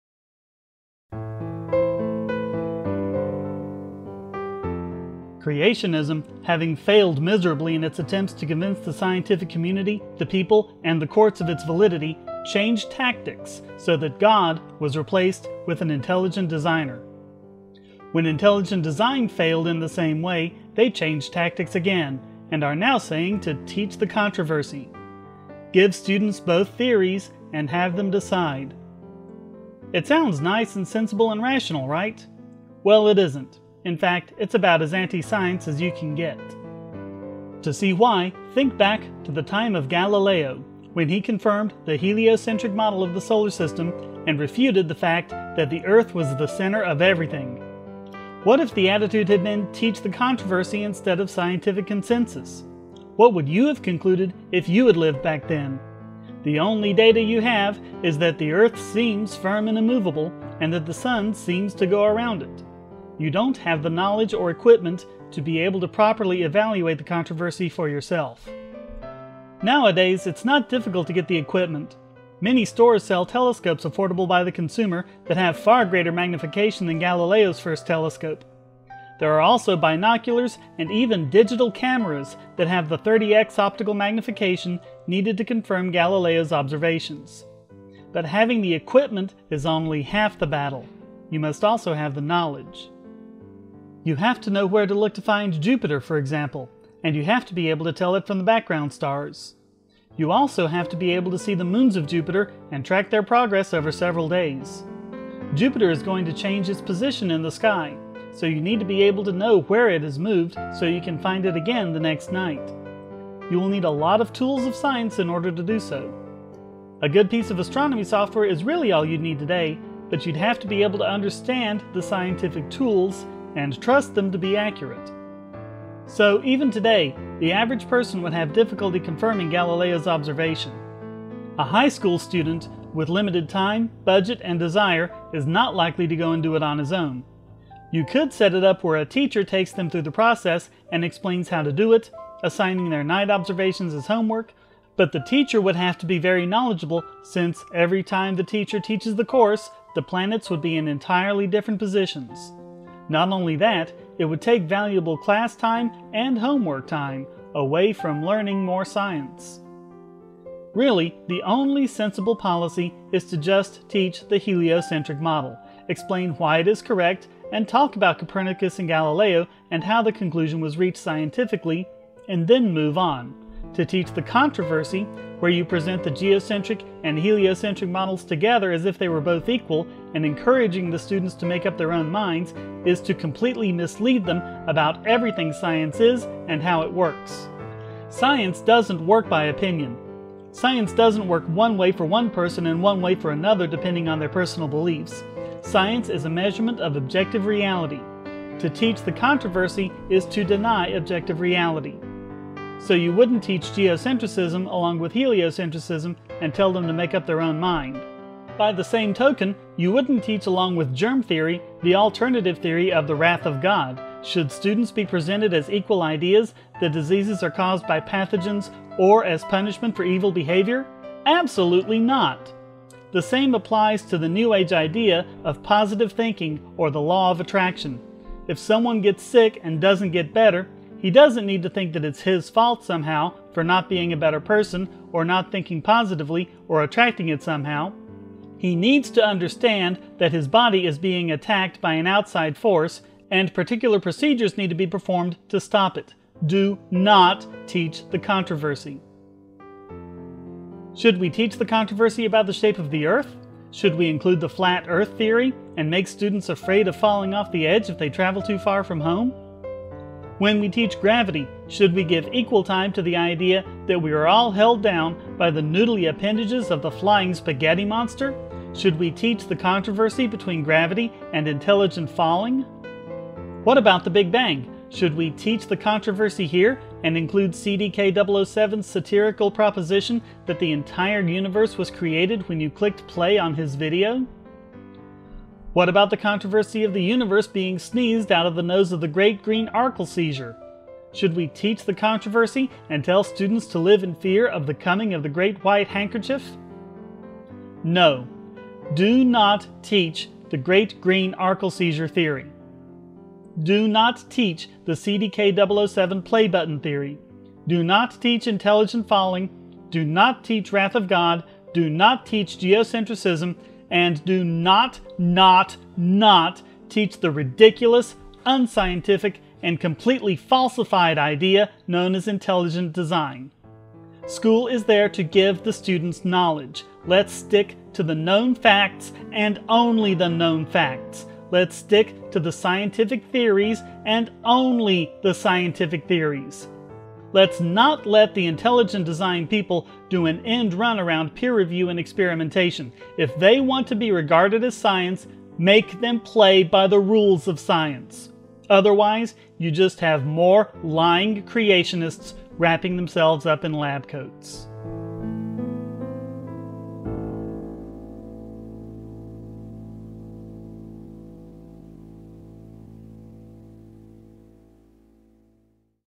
Should We Teach the Controversy? HD version
Another HD version of a previous video.

Now, the creationist/ID people say we should teach the "controversy" about evolution vs. intelligent design and let the students decide for themselves. It sounds very reasonable, but is it, really?